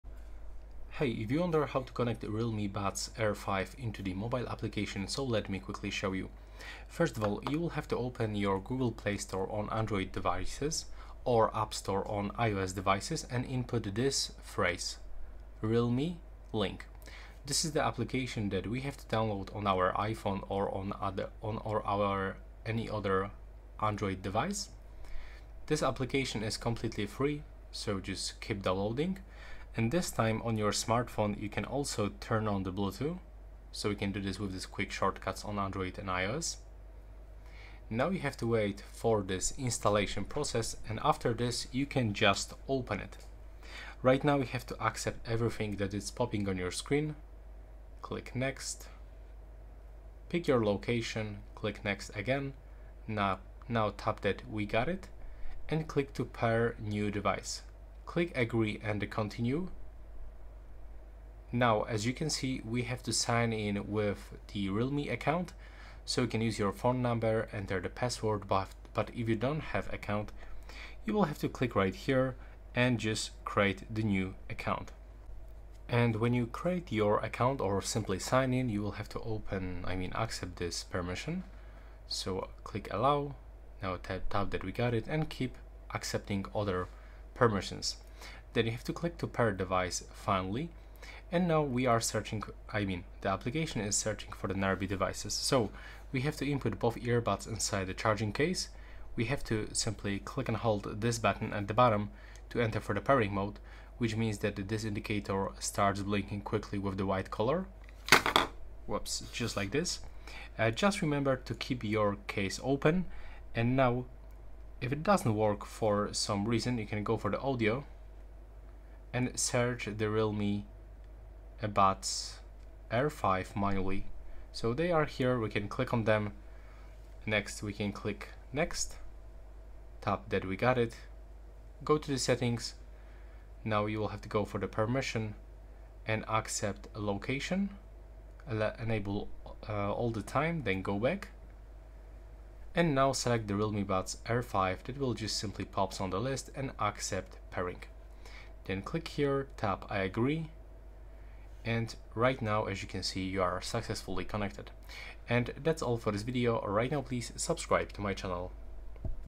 Realme Buds Air 5: Connect with App
Having trouble connecting your Realme Buds Air 5 to your smartphone? This video offers a step-by-step guide on pairing your earbuds with the Realme Link app.  In no time, you'll be enjoying your music and calls with ease!

If this video tutorial for the Realme Buds Air 5 wasn't clear enough and you still have questions, please leave them in the comments. I'll do my best to respond as soon as possible.